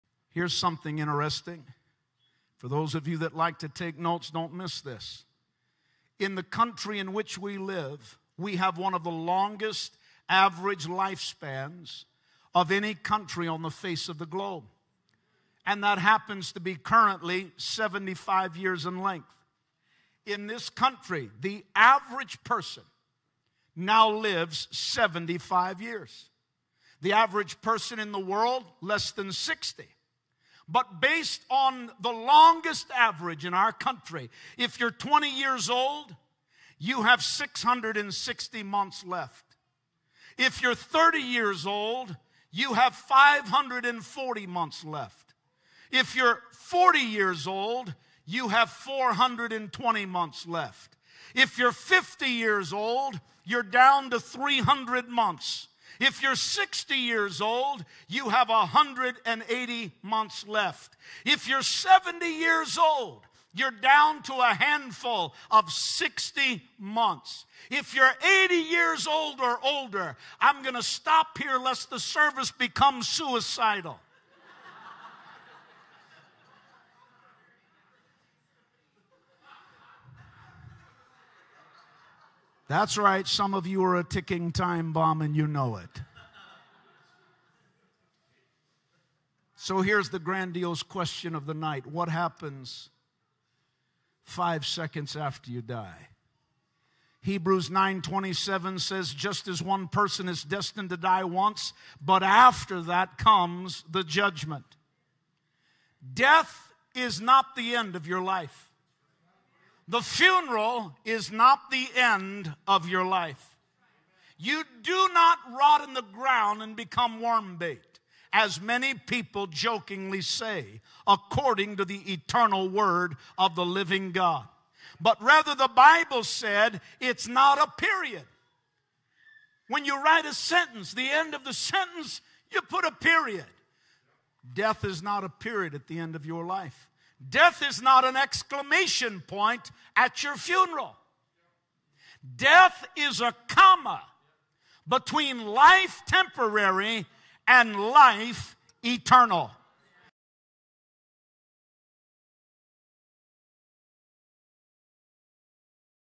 What will happen to you when you die?
🙌🔥The Bible is not silent when it comes to talking about what will happen to us after we die. The Word of God makes it very clear that death is not the final part in our lives, but that it is rather only a change in location. We will either go to heaven or hell. The Bible tells us that after death, we will be judged by God! So if you haven't already, receive Christ in your heart today, because you do not know when you are going to die and your decisions of today, will determine where you will spend your eternity!🔥🙌

Chapters:
0:00 Death is certain!
1:30 What happens after you die?
1:46 Death is not the end!
2:30 Outro